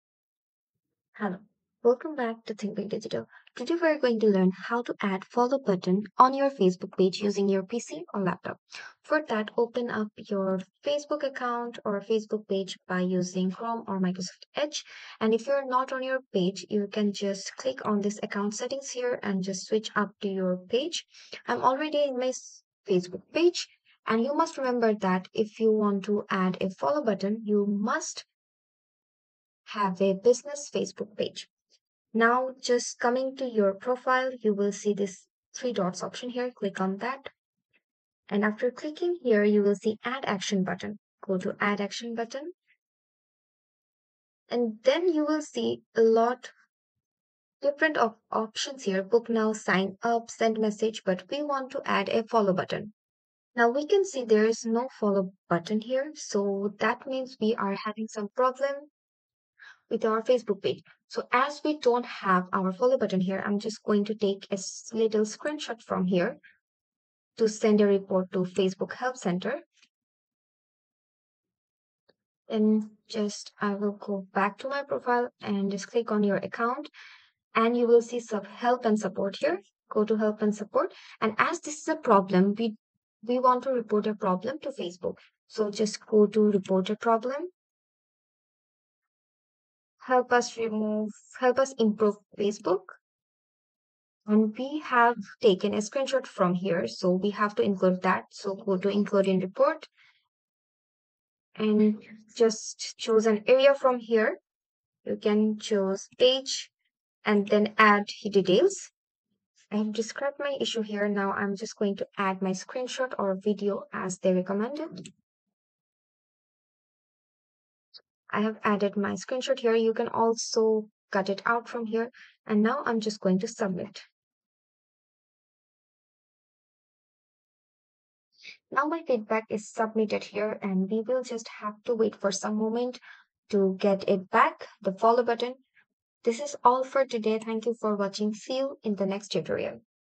How to Add Follow Button on Facebook Page in 2024
In this video I show you How to Add Follow Button on Facebook Page in PC in 2024